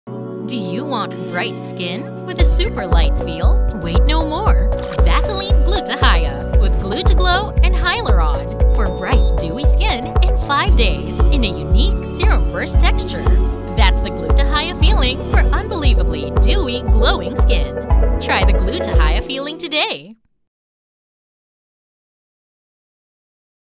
Bright Skin? Super Light Feel? Try the GlutaHyaFeeling with Vaseline Gluta-Hya Serum Burst Lotions!
Want bright skin but hate the heavy sticky feeling of lotions? Vaseline Gluta-Hya Serum Burst Lotions have a unique serum burst texture that bursts into water droplets as you apply to skin for bright skin with a super light feel!

Unbelievable Dewy Glow without the sticky feeling. That’s the Vaseline GlutaHyaFeeling! Try yours today!

ASC Reference Code: U0048P081723V